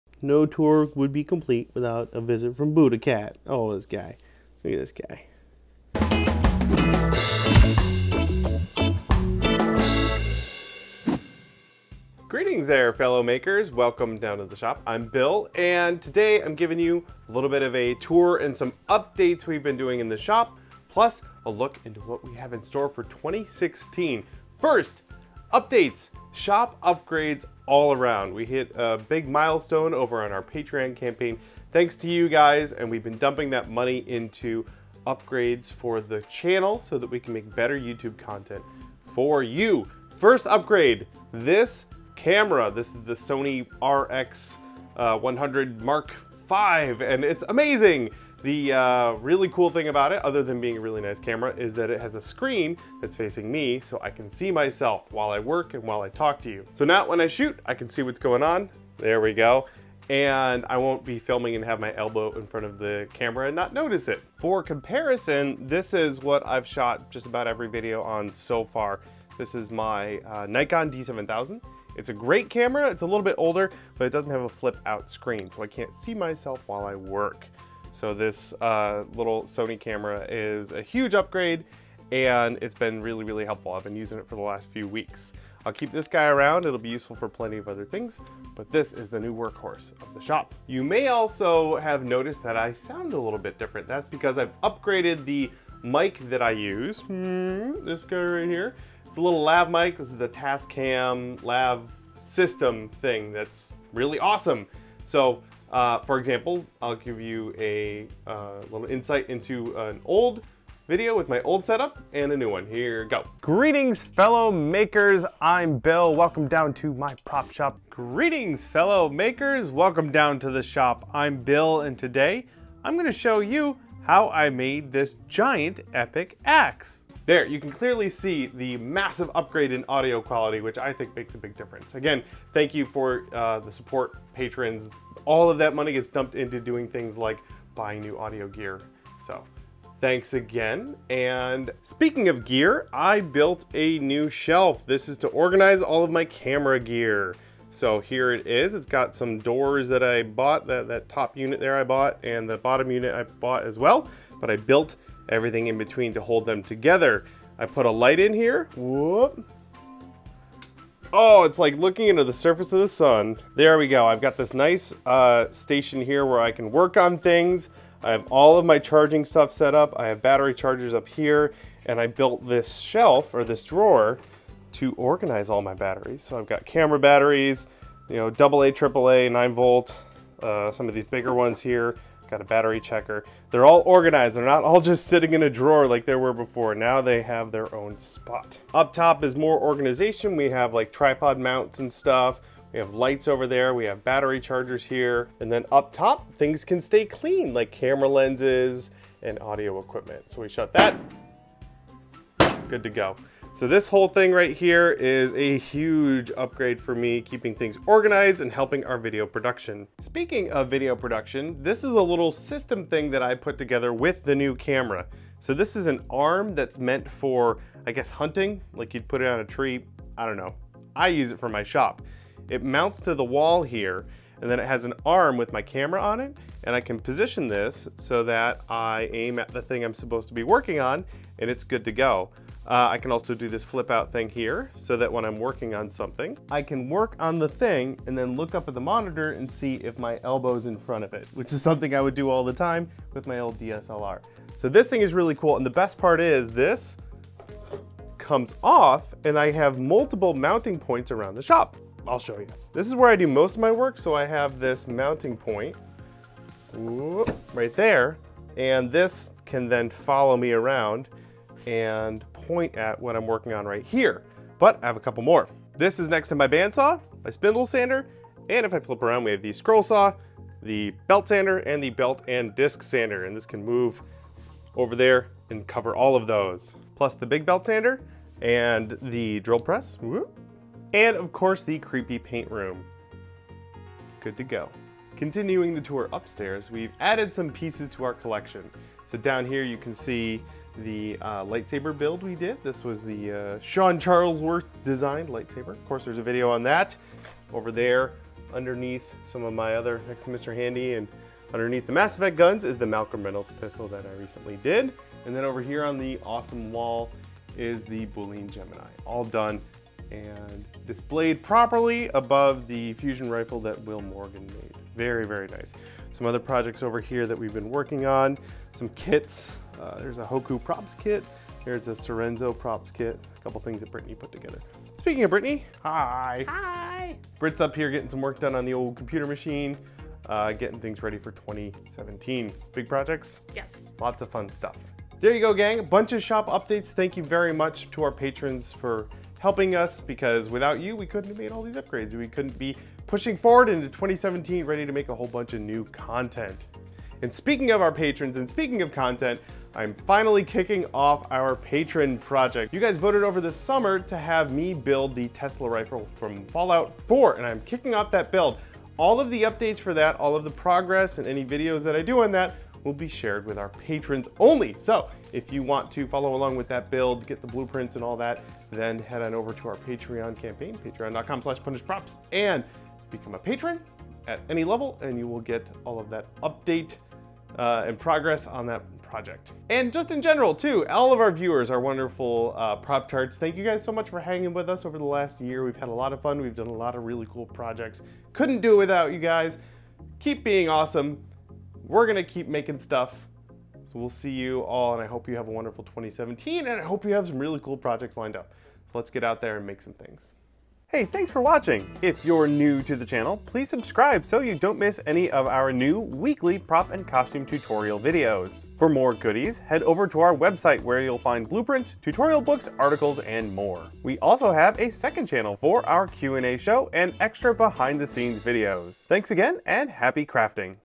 2017 Punished Props Shop Upgrades and New Year Hype
Thanks to the generous support from our incredible Patrons, we've done a bunch of equipment upgrades in the shop.

Tascam DR-10L Lav Mic Recorder

Camera Arm Mount